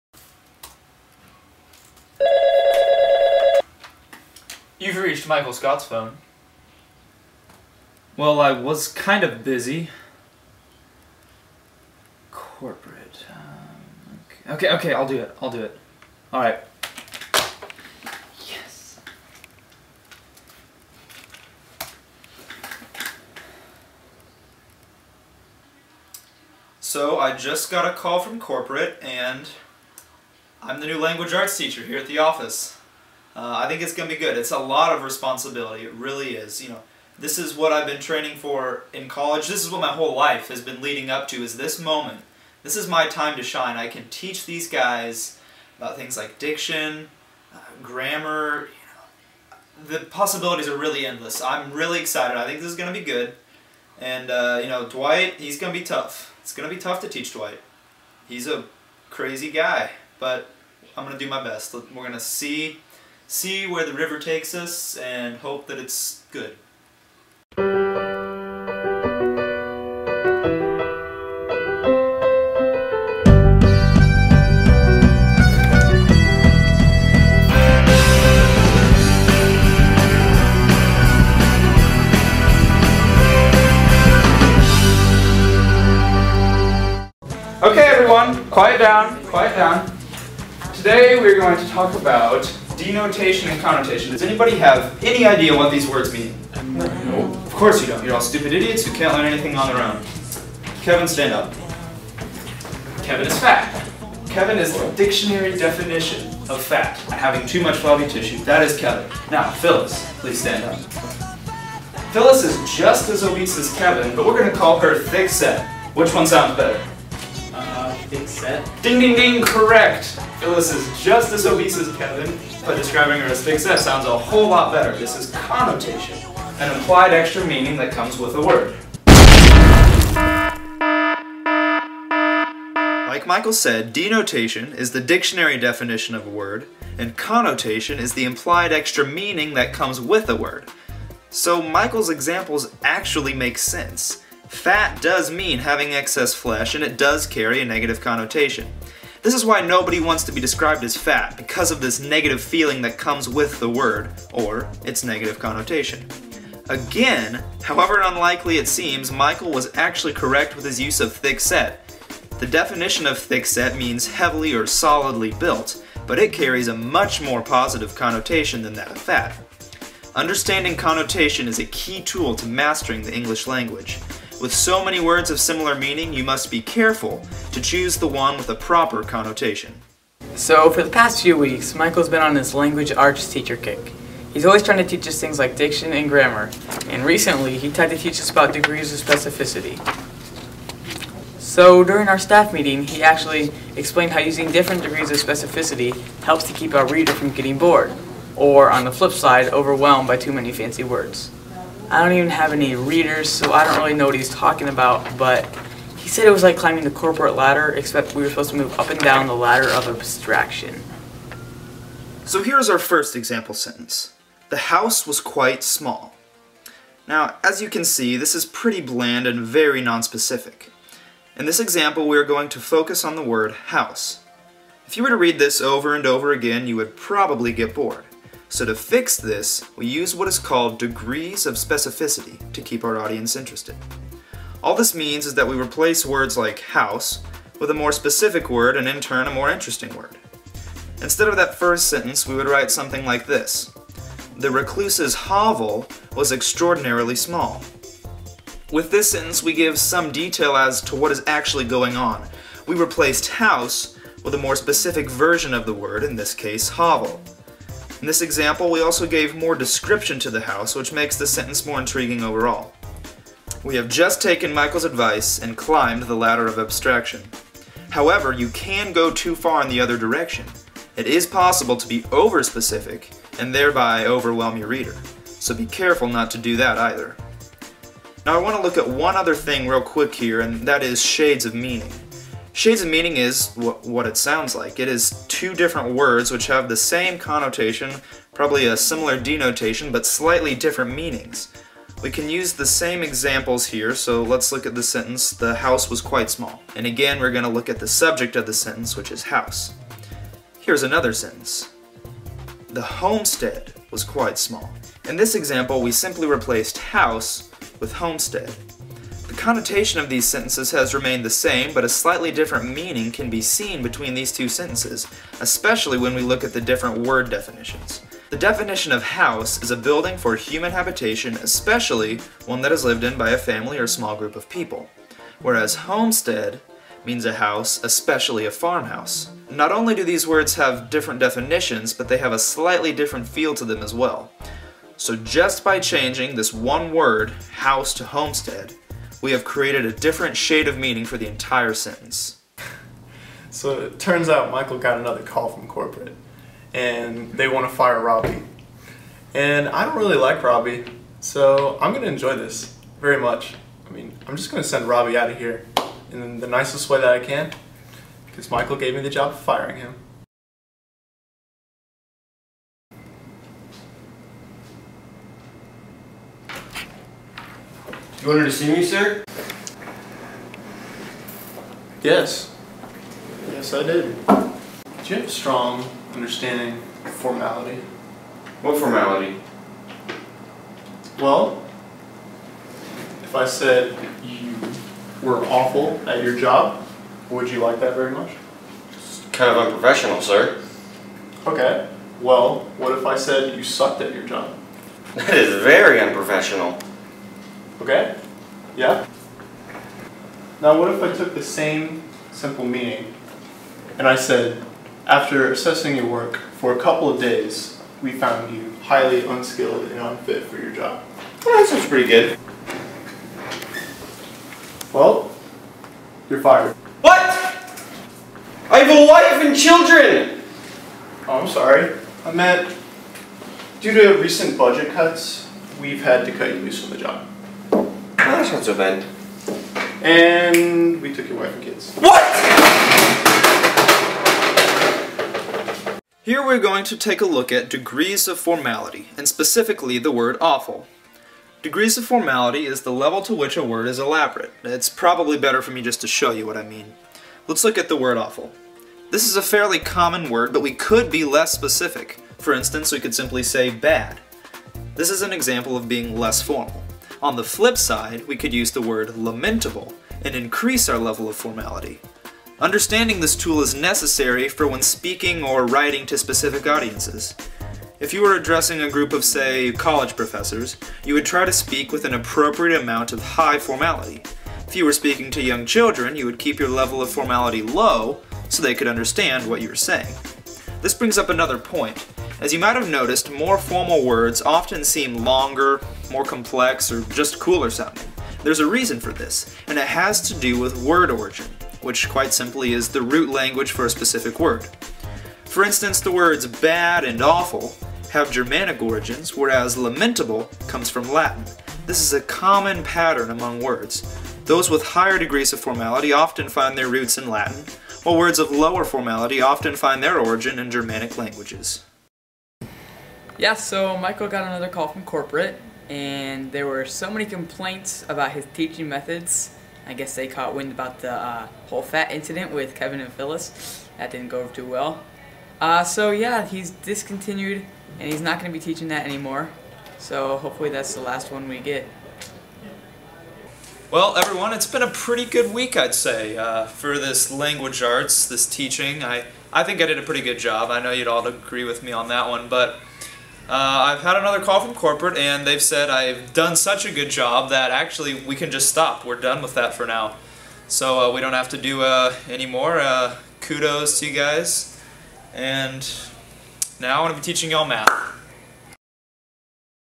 The Office Parody Language Project - BrianCarter
For my AP Language class, we had to do a project in which we taught about diction.  My group decided to do some office spoof scenes along with instructional video segments which we affectionately call "whiteboard scenes".  Enjoy!